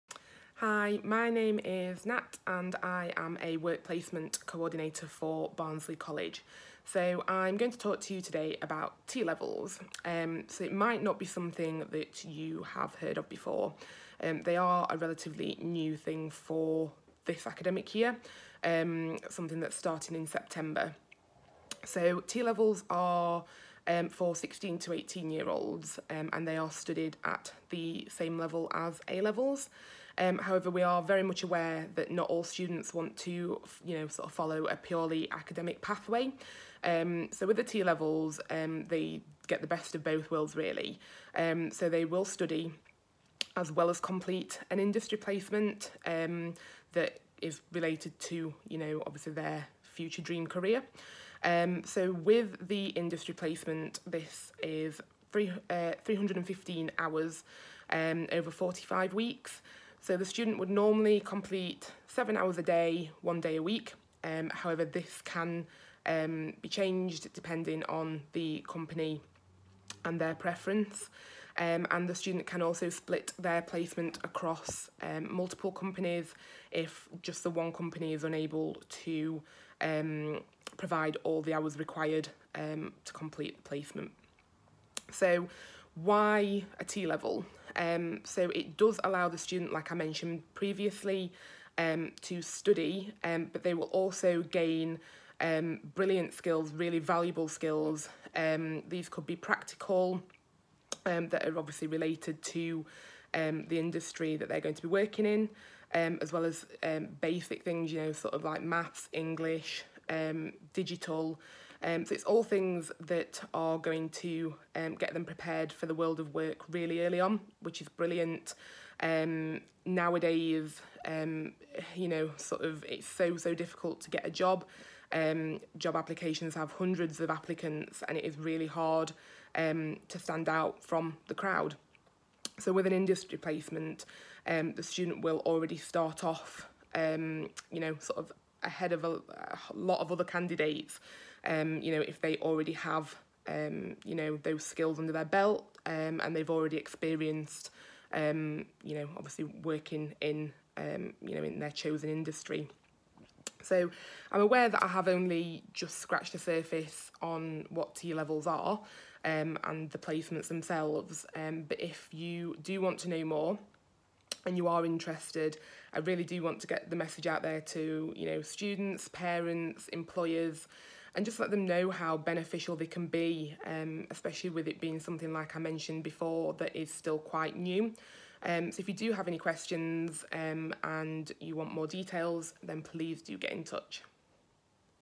Introduction to T Levels From a Newbie at Barnsley College!
If you're interested in T Levels and Industry Placements but don't really know anything about them or where to start looking, watch this video for a brief introduction. Please comment if you want any further details!